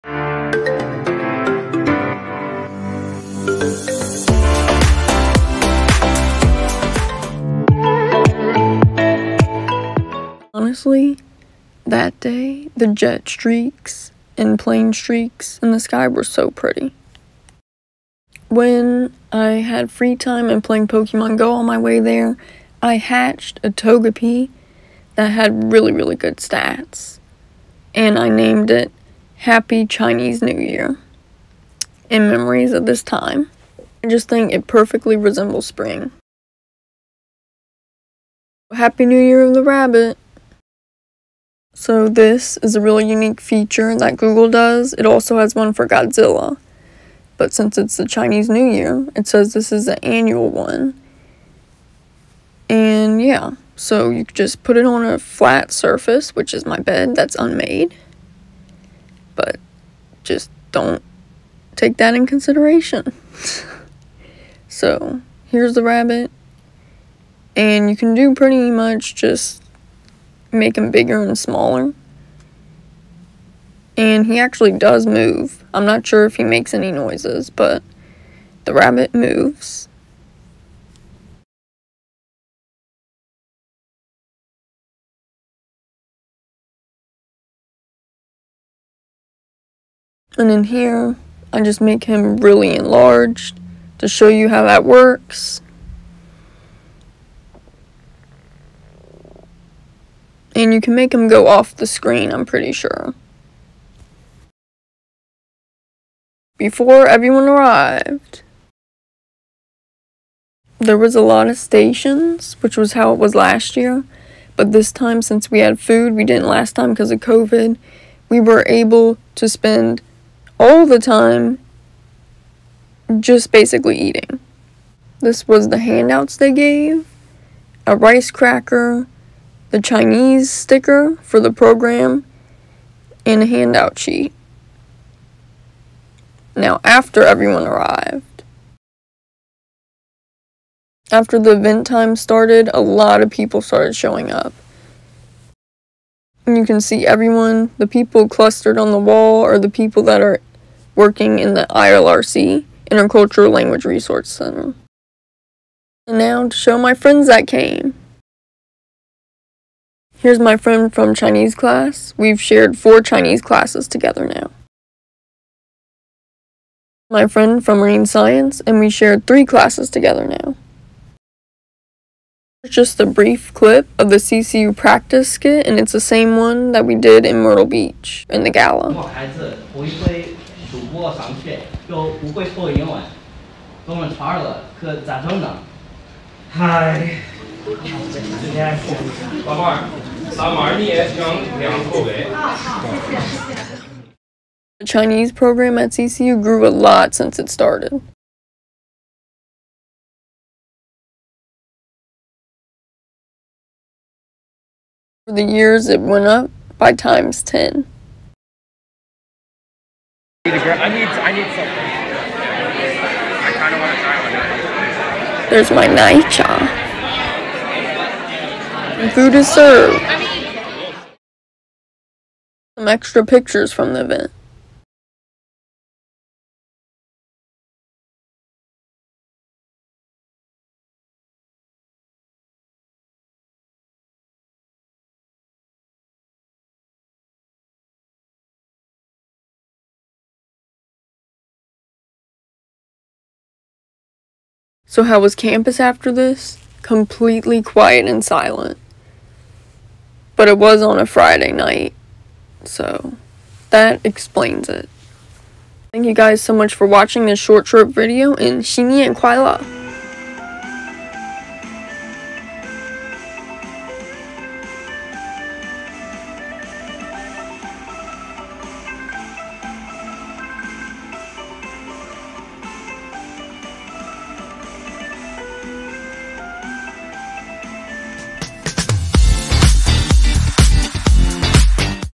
Coastal Carolina University 2023 Chinese New Year Celebration
Hello everyone!
Happy new year of the rabbit!
兔年行大運!

This event takes place each year at CCU, hope you enjoyed this very short video, and if you liked this one make sure to check out the Myrtle Beach New Year Gala video showing the full skit of the CCU students!